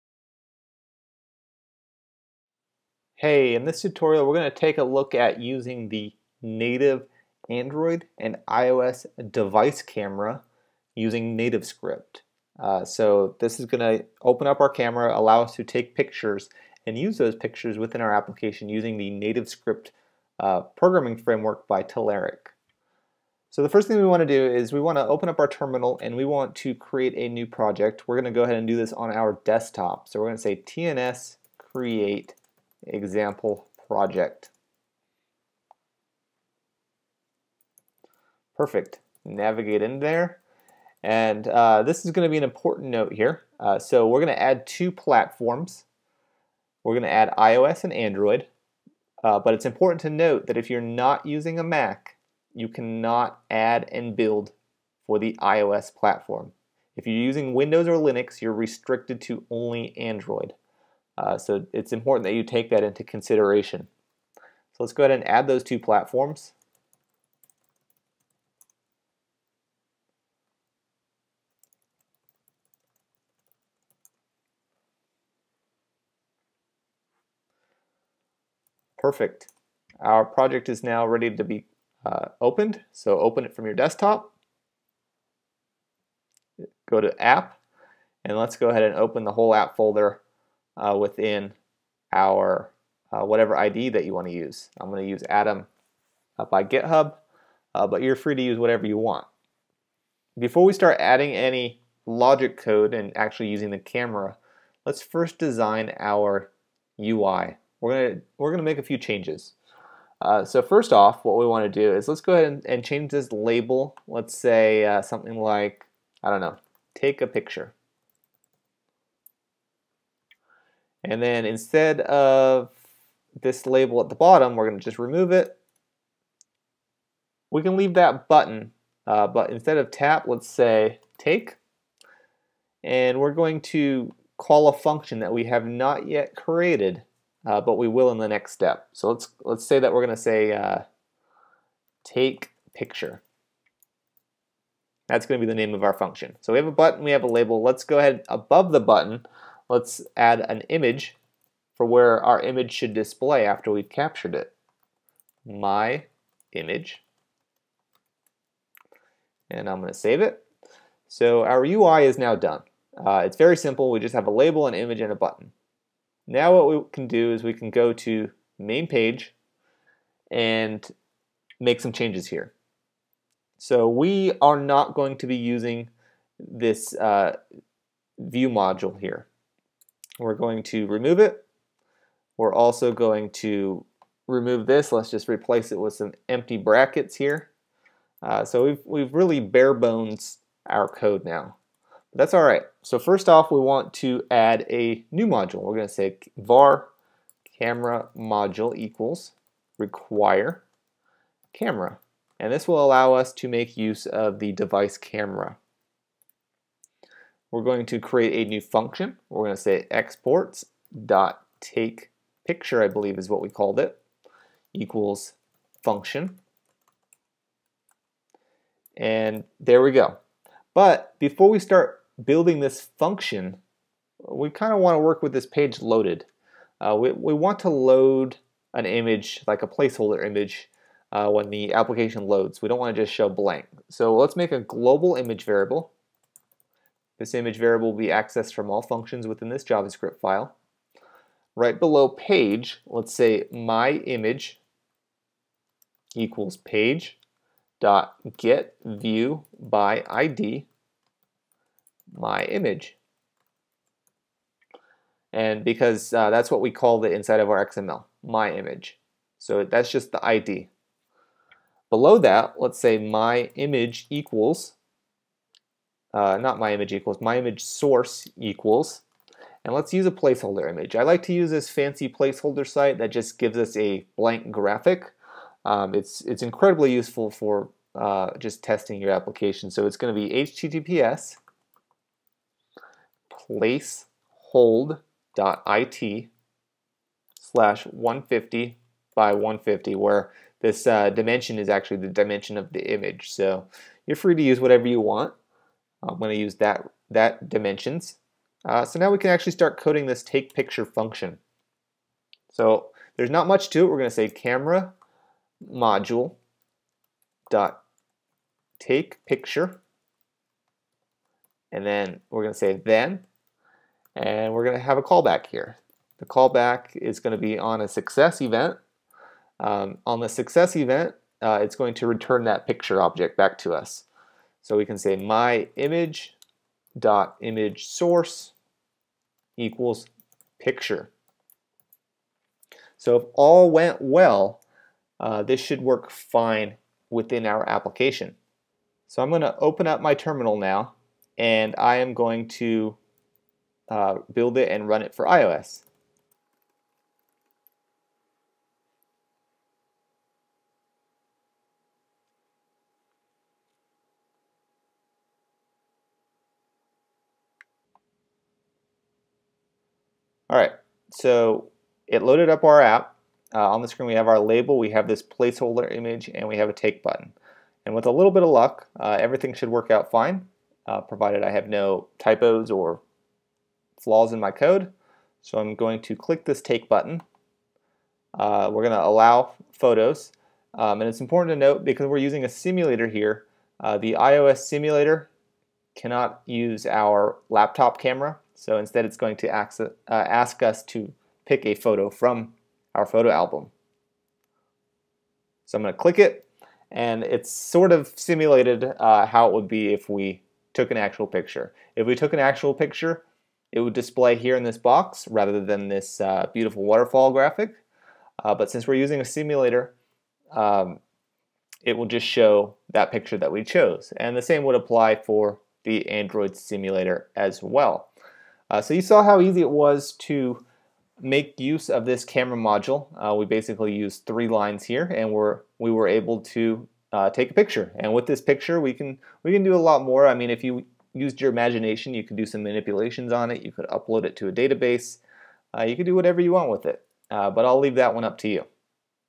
Using The Device Camera In Your NativeScript Mobile App
Access the native device camera using JavaScript in your NativeScript Android and iOS mobile application. Display images in your app or upload them.